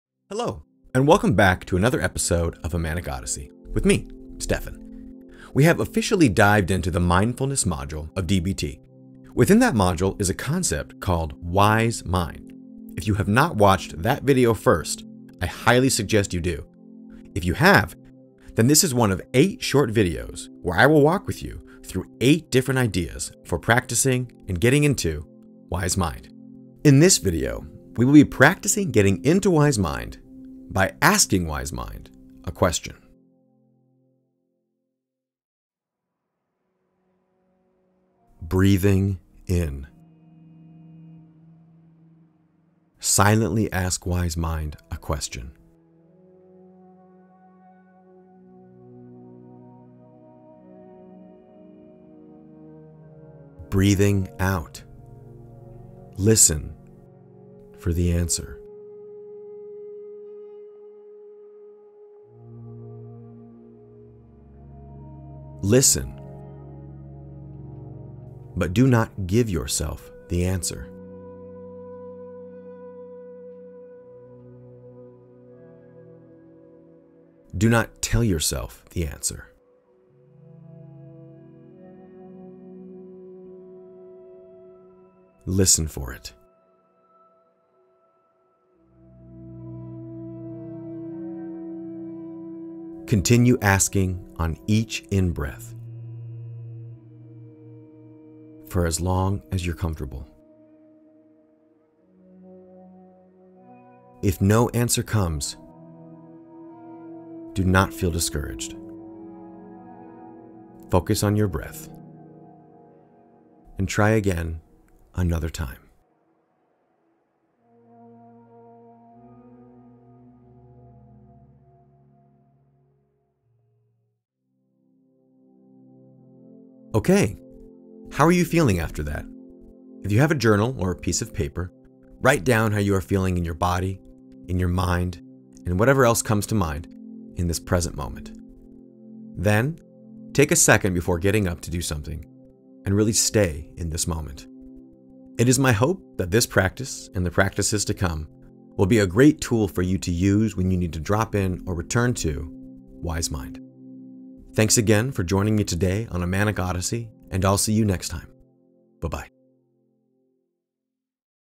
Episode 5.4: Mindfulness Practice: Asking Wise Mind A Question
This is one of eight quick mindfulness practices to help someone get into, or return to, Wise Mind.